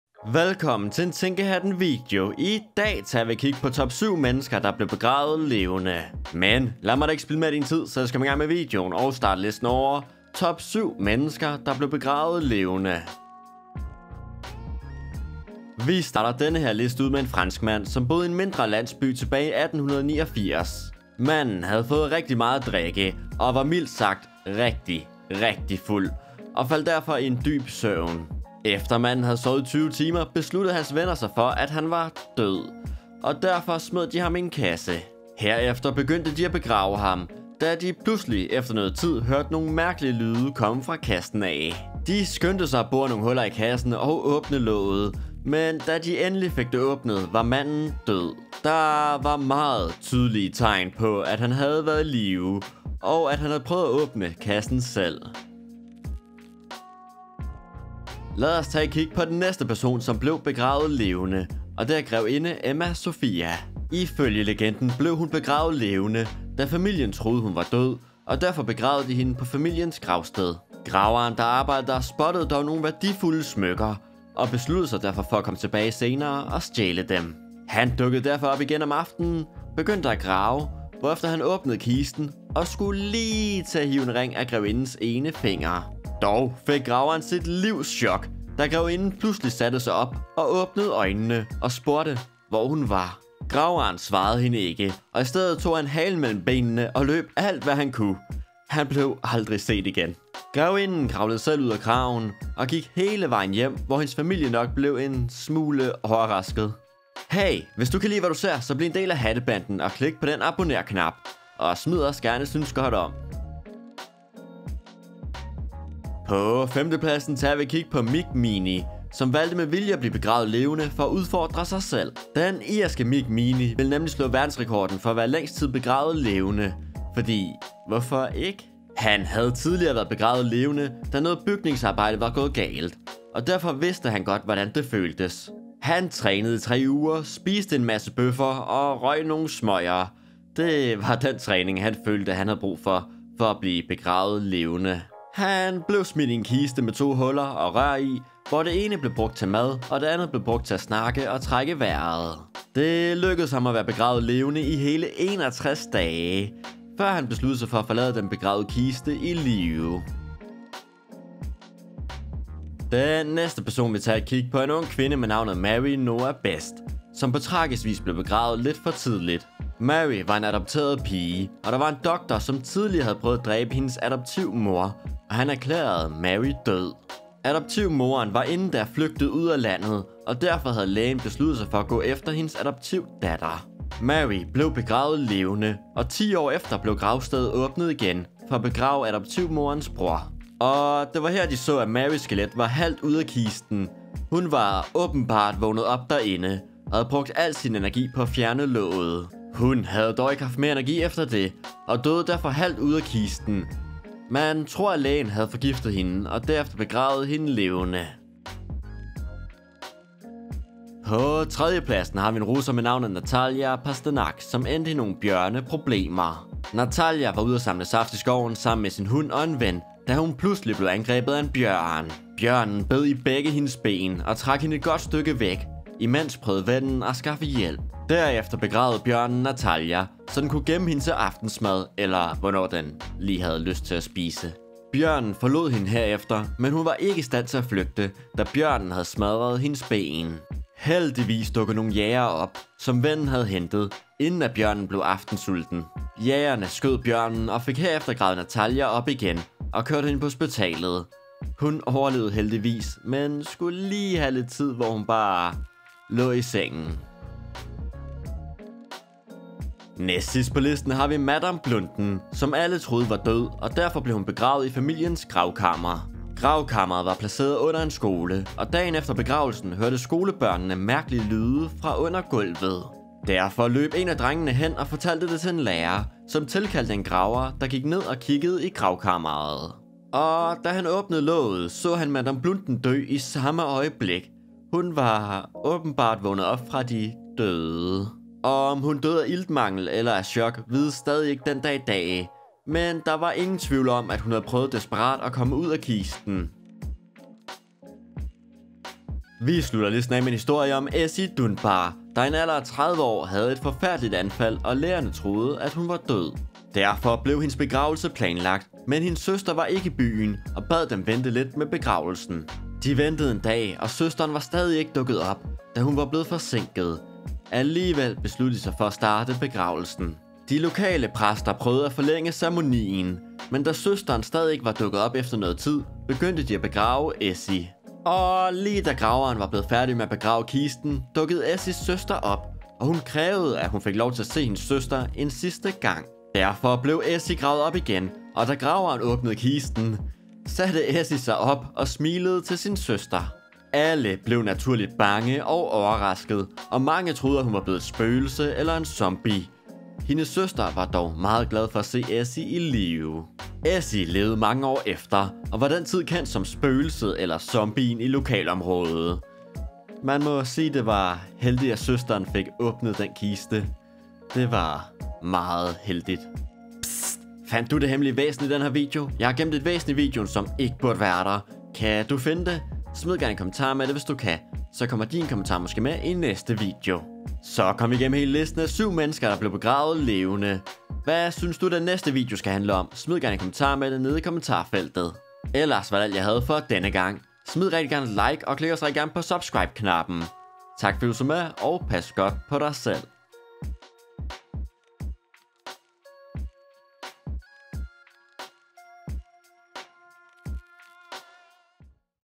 TOP 7 Mennesker Der Blev Begravet Levende!
Velkommen til en TænkeHatten video, og denne gang tager vi et kig på TOP 7 Mennesker Der Blev Begravet Levende! Rigtig mange mennesker bliver begravet hver dag, men i fortiden var mange af dem stadig i live. Der er ingen tvivl om at det må være en skræmmende oplevelse!
Lyst til mere TænkeHatten? Tjek Infohat ud!